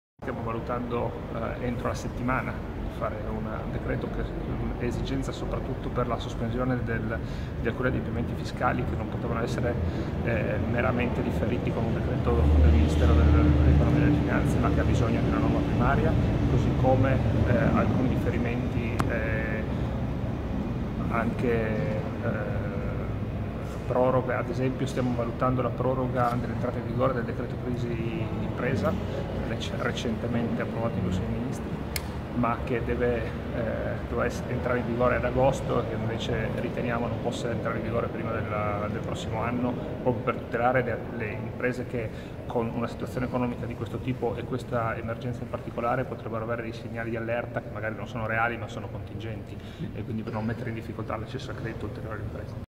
Coronavirus, misure per le imprese: rinviato il DL
La conferma del Ministro Pattuanelli